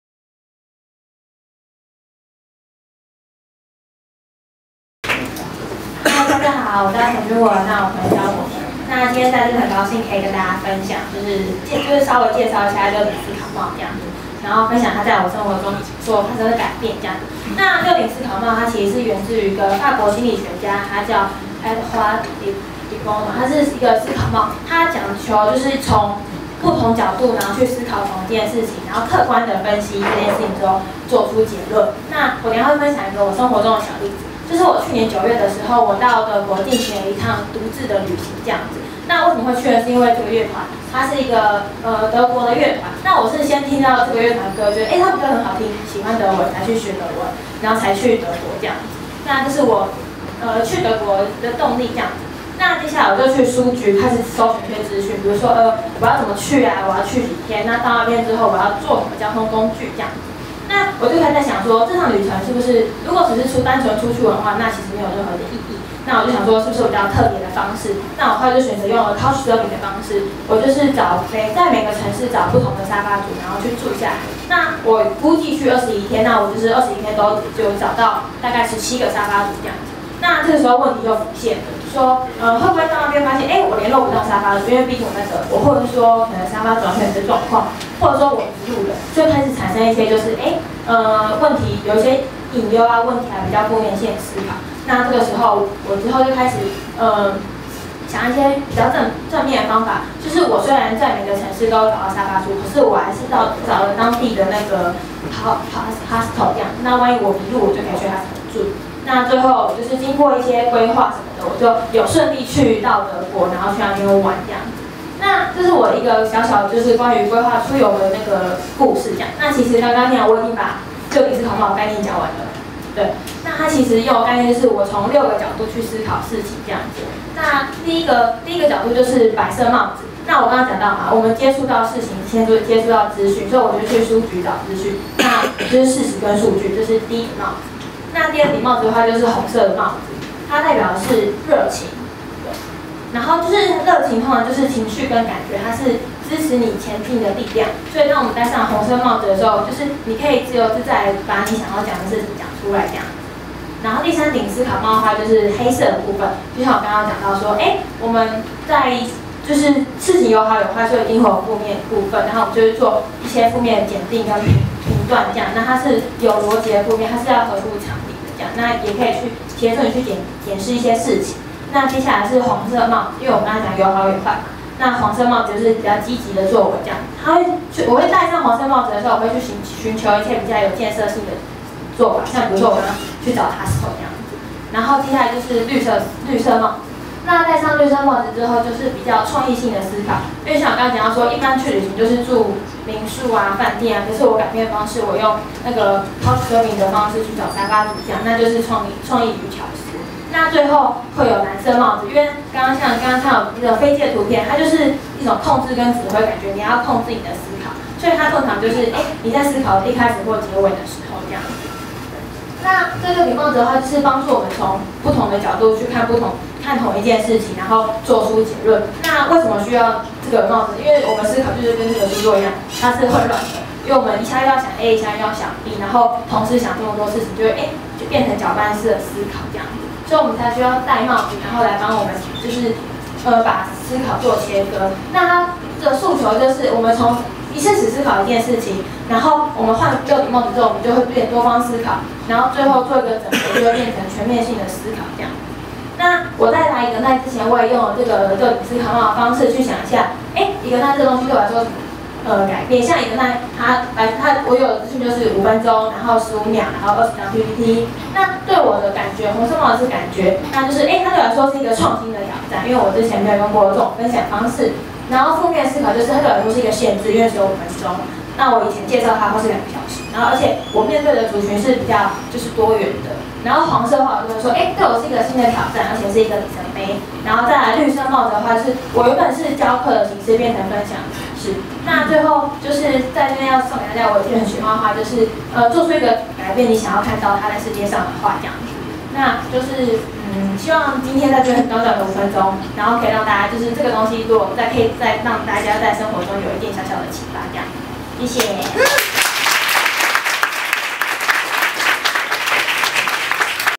陳君文: 神奇的思考帽 (Six Thinking Hats)
以去年九月至德國旅行（一個人的沙發客之旅）為故事主線，於故事之中置入六頂思考帽，向大家介紹六頂思考帽的功能，進而分享思考帽在生活之中的影響。

Talk originally at Ignite Taipei #8 on 2013 March 3.
Video editing by Gergely Imreh